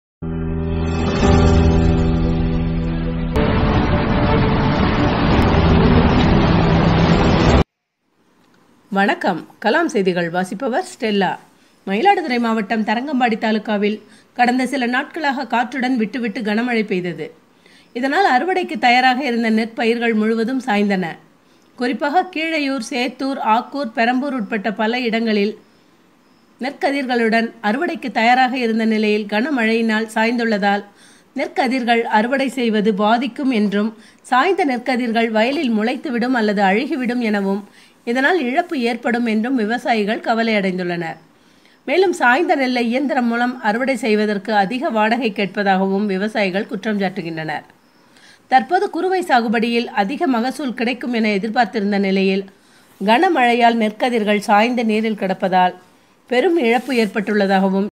KALAM NEWS தரங்கம்பாடி தாலுக்காவில் கனமழை அறுவடைக்கு தயாராக இருந்த நெற்பயிர்கள் முழுவதும் சாய்ந்தன.
KALAM NEWS
தரங்கம்பாடி தாலுக்காவில்  கனமழை அறுவடைக்கு தயாராக இருந்த நெற்பயிர்கள் முழுவதும் சாய்ந்தன.